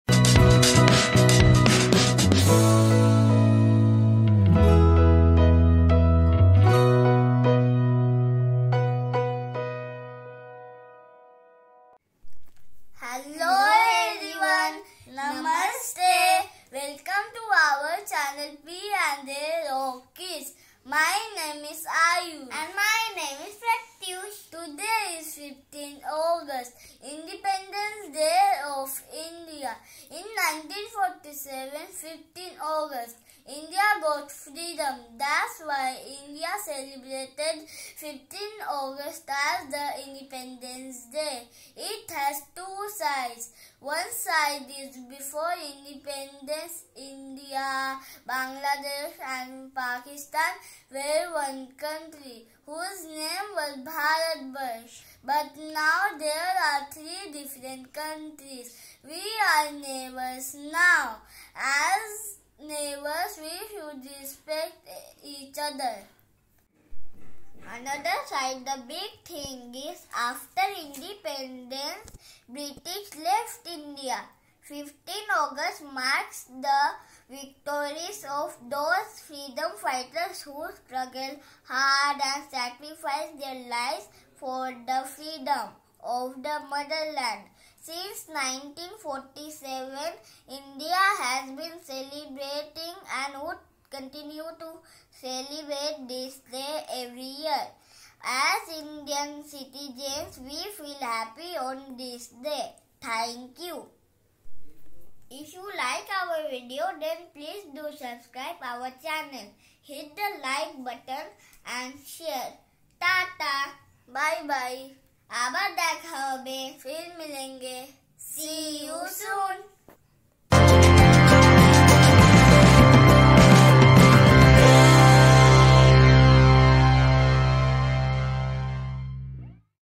About Independence Day of India | About 15 August
In this channel we shall make different types of videos in Bengali, Hindi and English.

**In this video we have spoken on Independence Day of India.

Many many thanks for giving us your valuable time and watching our video.